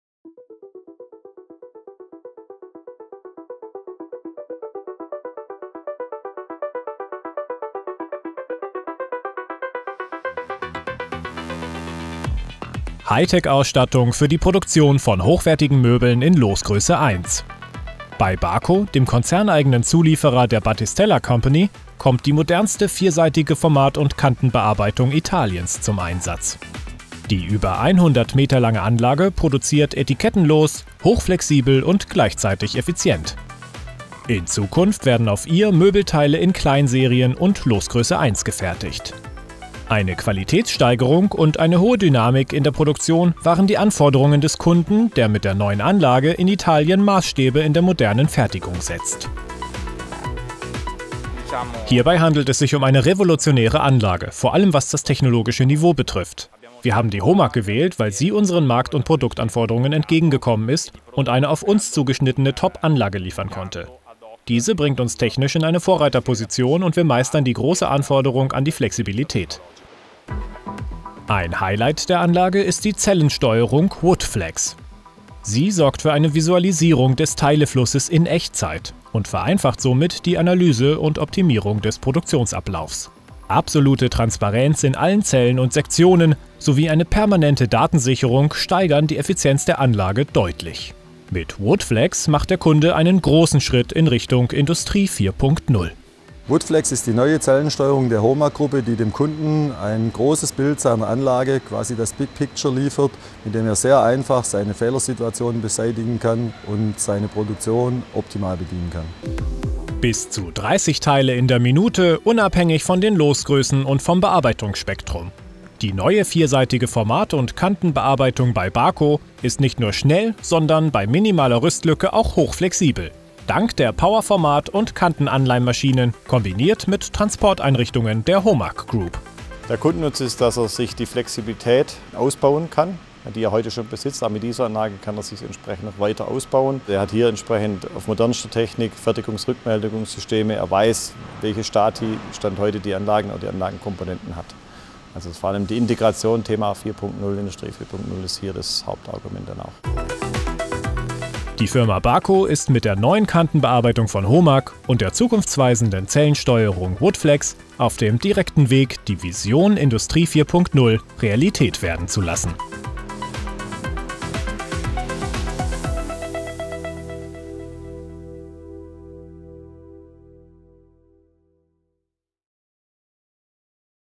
Battistella Company: Hochwertige Möbel in Losgröße 1 mit Zellensteuerung woodFlex
Hightech-Ausstattung für die Produktion von hochwertigen Möbeln in Losgröße 1.
Bei Baco, dem konzerneigenen Zulieferer der Battistella Company, kommt die modernste vierseitige Format- und Kantenbearbeitung Italiens zum Einsatz.
Die über 100 Meter lange Anlage produziert etikettenlos, hochflexibel und gleichzeitig effizient.
In Zukunft werden auf ihr Möbelteile in Kleinserien und Losgröße 1 gefertigt.
Eine Qualitätssteigerung und eine hohe Dynamik in der Produktion waren die Anforderungen des Kunden, der mit der neuen Anlage in Italien Maßstäbe in der modernen Fertigung setzt.